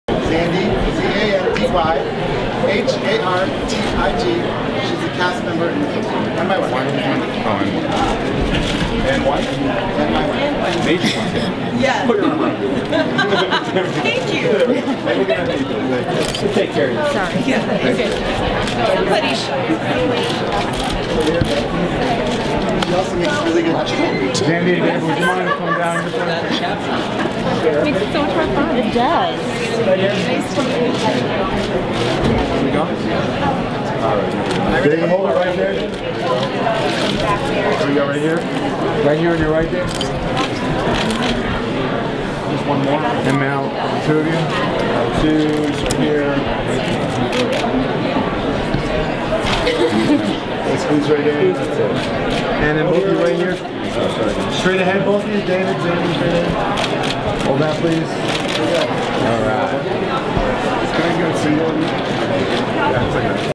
They Came Together: David Wain and Wife red carpet
They Came Together: David Wain and Wife red carpet pose for photos